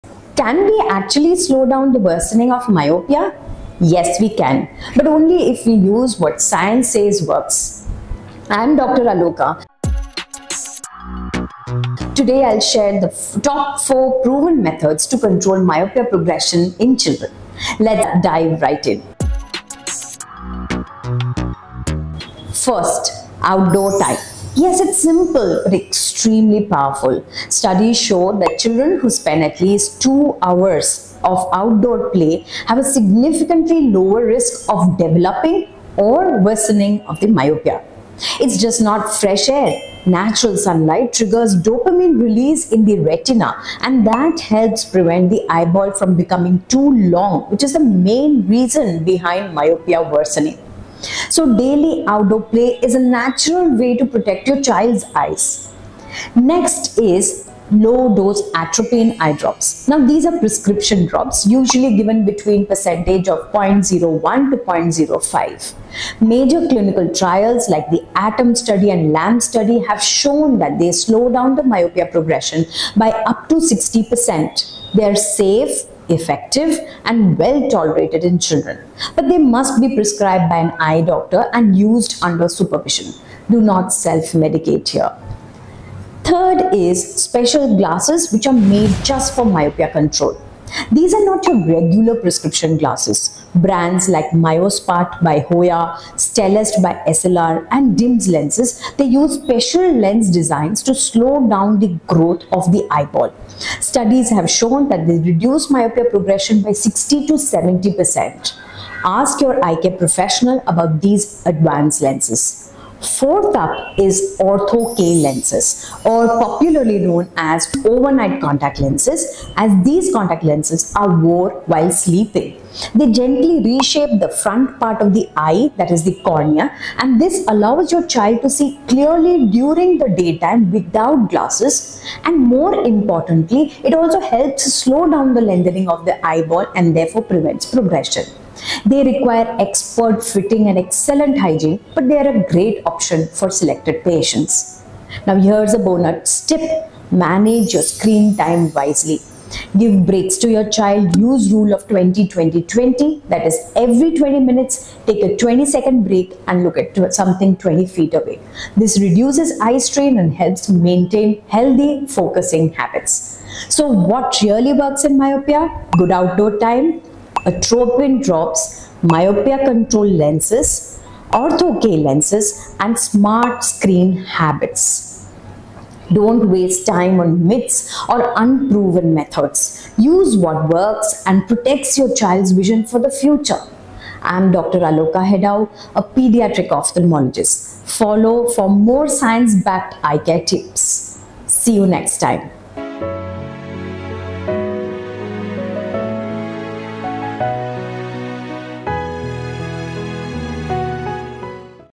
What Works to Prevent Myopia Progression
👁️ What Actually Works to Prevent Myopia Progression in Children?
As a Pediatric Ophthalmologist, I’m often asked how to slow down the worsening of nearsightedness (myopia) in kids. In this video, I break down evidence-based strategies that have been proven to work—and also clarify what doesn’t help.

✅ What You'll Learn:

Role of outdoor time in preventing myopia

How low-dose atropine drops work

Effectiveness of specialized myopia control glasses and contact lenses

Importance of limiting screen time & near work

What’s backed by science—and what’s just a myth

🔍 Whether you're a concerned parent, educator, or healthcare professional, this video will guide you through real, practical solutions to protect your child’s vision.